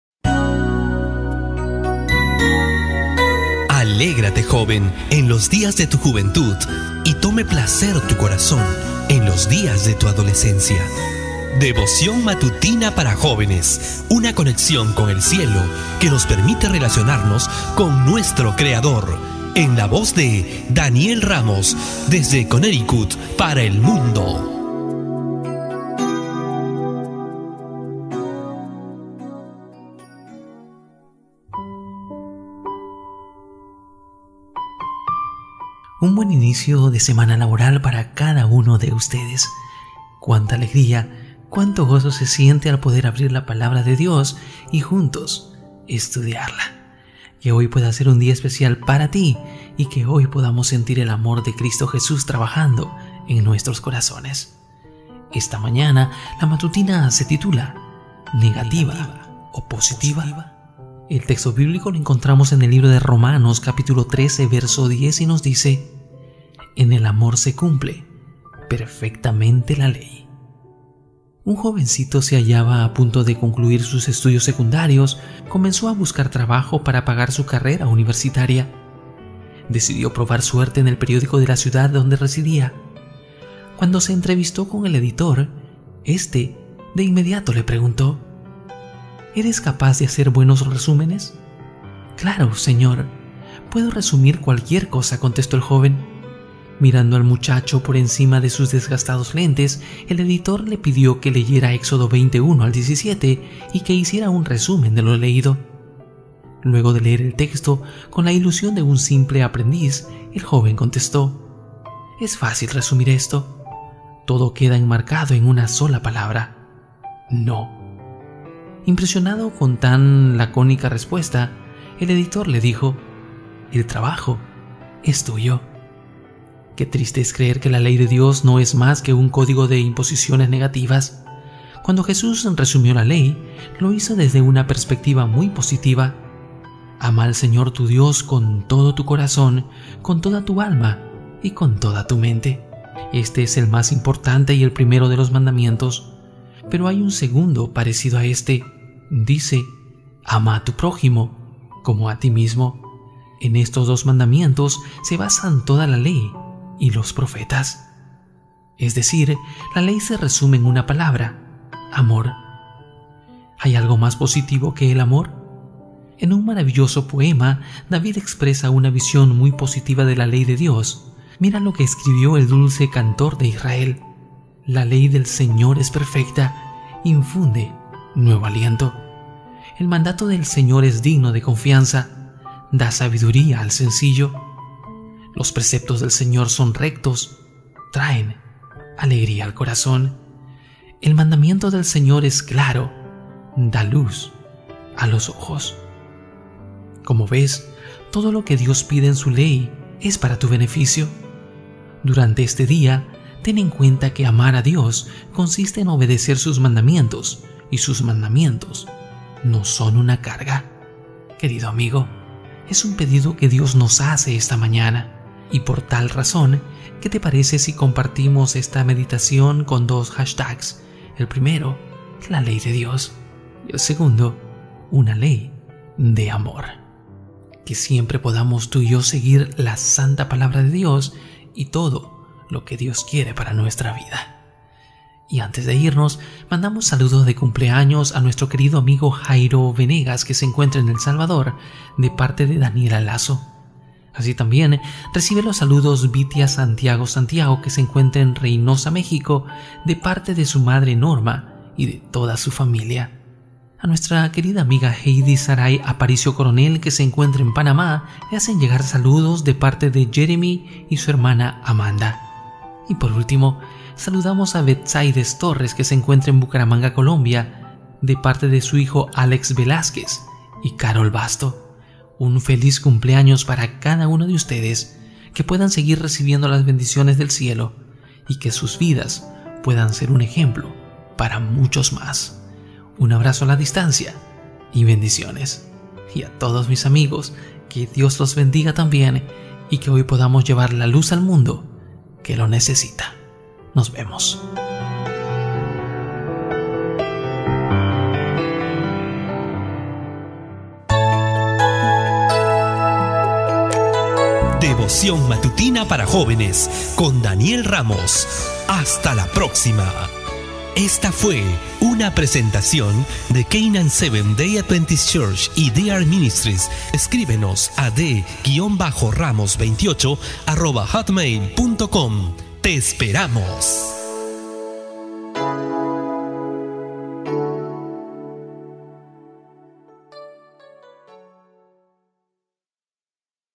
Devoción Matutina Para Jóvenes Lunes 02 de Mayo del 2016
¿NEGATIVA O POSITIVA?

“En el amor se cumple perfectamente la ley” (Romanos 13:10).

Un jovencito que se hallaba a punto de concluir sus estudios secundarios comenzó a buscar trabajo para pagar su carrera universitaria. Decidió probar suerte en el periódico de la ciudad donde residía. Cuando se entrevistó con el editor, este de inmediato le preguntó:

-¿Eres capaz de hacer buenos resúmenes?

-Claro, señor. Puedo resumir cualquier cosa -contestó el joven.

Mirando al muchacho por encima de sus desgastados lentes, el editor le pidió que leyera Éxodo 20:1 al 17 y que hiciera un resumen de lo leído.

Luego de leer el texto, con la ilusión de un simple aprendiz, el joven contestó: -Es fácil resumir esto. Todo queda enmarcado en una sola palabra: No. Impresionado con tan lacónica respuesta, el editor le dijo: “El trabajo es tuyo”. ¡Qué triste es creer que la ley de Dios no es más que un código de imposiciones negativas! Cuando Jesús resumió la ley, lo hizo desde una perspectiva muy positiva: “ ‘Ama al Señor tu Dios con todo tu corazón, con toda tu alma y con toda tu mente’. Este es el más importante y el primero de los mandamientos. Pero hay un segundo, parecido a este; dice: ‘Ama a tu prójimo como a ti mismo’. En estos dos mandamientos se basan toda la ley y los profetas” (Mateo 22:37-40). Es decir, la ley se resume en una palabra: Amor. ¿Hay algo más positivo que el amor?

En un maravilloso poema, David expresa una visión muy positiva de la ley de Dios. Mira lo que escribió el dulce cantor de Israel:

“La ley del Señor es perfecta: infunde nuevo aliento.

El mandato del Señor es digno de confianza: da sabiduría al sencillo.

Los preceptos del Señor son rectos: traen alegría al corazón.

El mandamiento del Señor es claro: da luz a los ojos” (Salmo 19:7, 8, NVI). Como ves, todo lo que Dios pide en su ley es para tu beneficio. Durante este día ten en cuenta que “amar a Dios consiste en obedecer sus mandamientos; y sus mandamientos no son una carga” (1 Juan 5:3).

Visita mi muro
Por: J. Vladimir Polanco